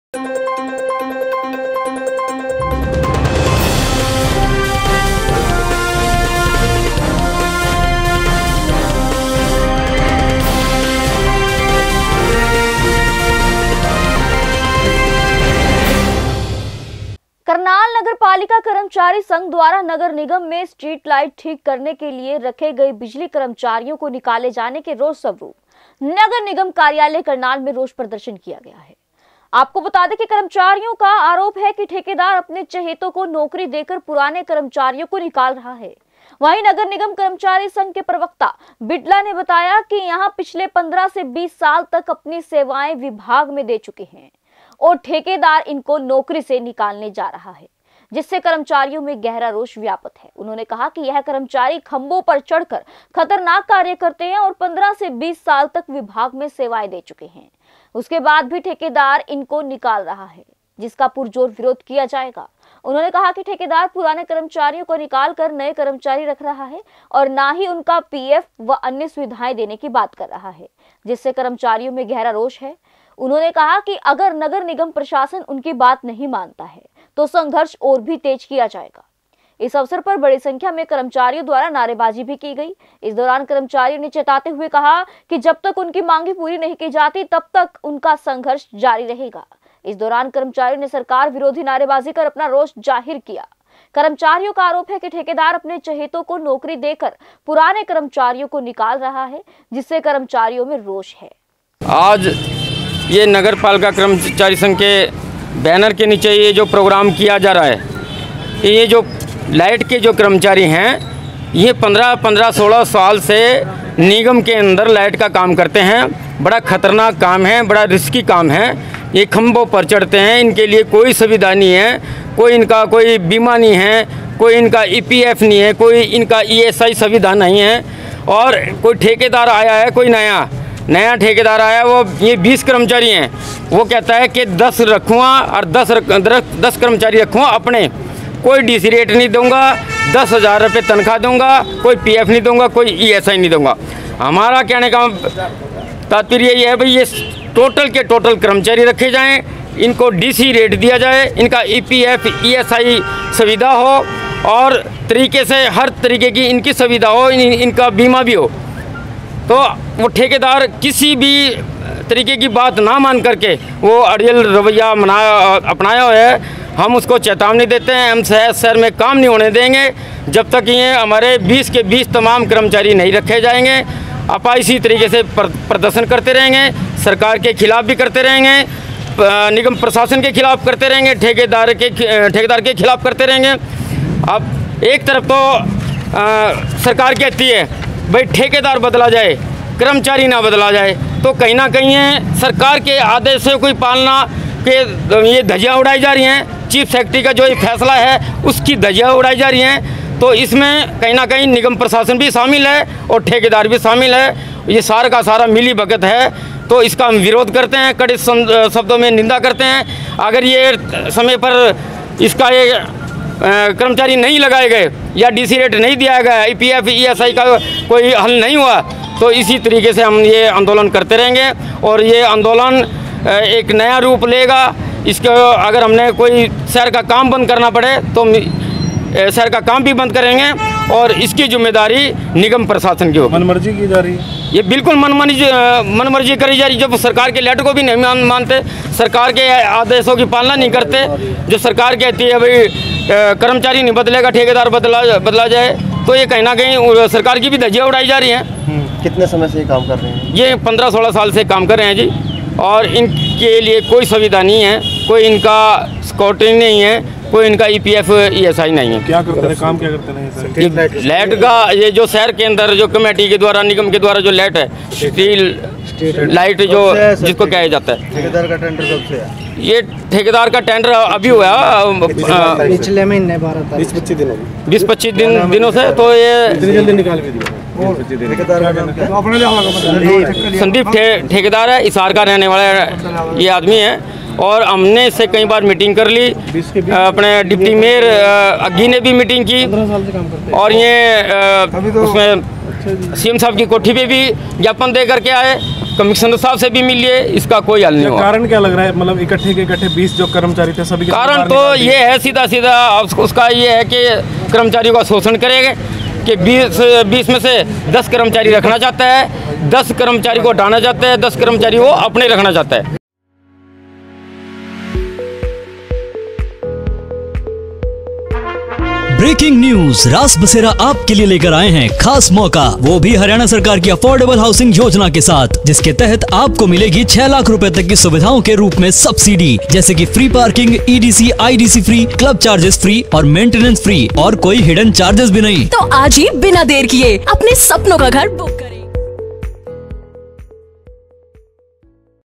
ठेकेदार अपने चहेतों को नौकरी देकर छीन रहा नौकरी
करनाल नगर पालिका कर्मचारी संघ द्वारा नगर निगम में स्ट्रीट लाइट ठीक करने के लिए रखे गए बिजली कर्मचारियों को निकाले जाने के रोषस्वरूप नगर निगम कार्यालय करनाल में रोष प्रदर्शन किया गया है। आपको बता दें कि कर्मचारियों का आरोप है कि ठेकेदार अपने चहेतों को नौकरी देकर पुराने कर्मचारियों को निकाल रहा है। वहीं, नगर निगम कर्मचारी संघ के प्रवक्ता बिड़ला ने बताया कि यहां पिछले 15 से 20 साल तक अपनी सेवाएं विभाग में दे चुके हैं